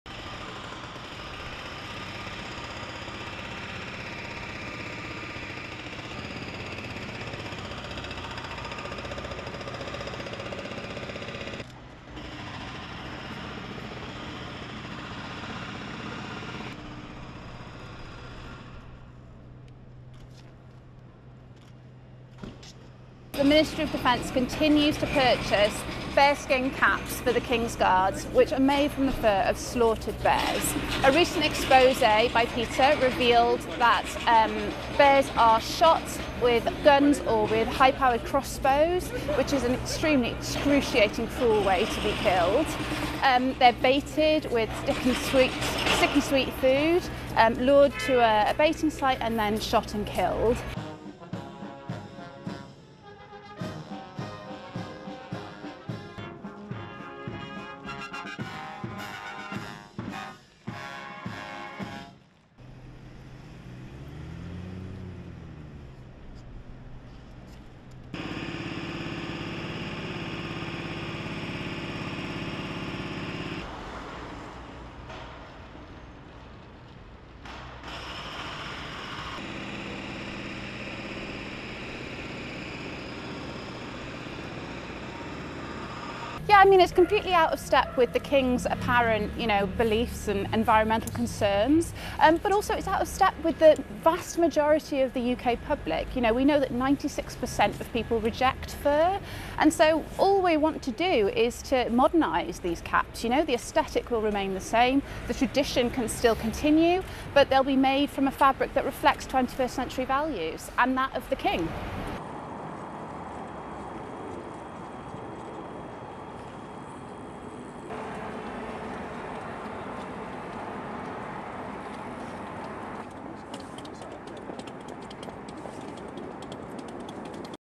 Animal rights group protest against the use of fur in iconic bearskin hats of UK King's Guard
RESTRICTION SUMMARY:

ASSOCIATED PRESS
London - 19 January 2024
1. Various of PETA (People for the Ethical Treatment of Animals) supporters protesting dressed in bear masks and covered in fake arrow wounds lying on Union Flag near the British Ministry of Defence's headquarters
2. Close of placard reading (English) "Shot With Crossbows! 1 Cap = 1 Bear MoD: Go Fur-Free"
3. SOUNDBITE (English) Kate Werner, Senior Campaign Manager for PETA UK:
"The Ministry of Defence continues to purchase bearskin caps for the King's Guards, which are made from the fur of slaughtered bears. A recent expose by PETA revealed that bears are shot with guns or with high powered crossbows, which is an extremely excruciating, cruel way to be killed. They're baited with sticky sweet food, lured to a baiting site and then shot and killed."

ASSOCIATED PRESS
London - 21 November 2023
4. Various of King's Guards marching

ASSOCIATED PRESS
5. Various of protest
6. SOUNDBITE (English) Kate Werner, Senior Campaign Manager for PETA UK:
"Yeah, I mean it's completely out of step with the king's apparent, you know, beliefs and environmental concerns. But also, it's out of step with the vast majority of the UK public. You know, we know that 96% of people reject fur. And so all we want to do is to modernize these caps, you know, the aesthetic will remain the same, the tradition can still continue, but they'll be made from a fabric that reflects 21st century values and that of the king."

ASSOCIATED PRESS
London, UK -  18 January 2024
7. Wide of Buckingham Palace
8. Various of King's Guard on duty
STORYLINE:
Protesters from the animal rights group PETA (People for the Ethical Treatment of Animals) UK gathered outside the British Defence Ministry in London on Friday as they called on the UK government to stop using real fur in the country's iconic bearskin caps for members of the King's Guard.

The protesters were dressed in bear masks and covered themselves in fake blood and arrow wounds as they laid on a Union Flag on the sidewalk.

Early this month, PETA released an undercover investigation video that it said showed hunters baiting black bears with food and shooting them with crossbows in Canada.

The video was narrated by actor and comedian Stephen Fry.

"All we want to do is to modernize these caps," said Kate Werner, senior campaign manager for PETA UK.

"The aesthetic will remain the same, the tradition can still continue, but they'll be made from a fabric that reflects 21st century values and that of the king," Werner added.

Britain's Defence Ministry said all the fur used in the hats comes from legal and licensed hunts.

AP video shot by Kwiyeon Ha.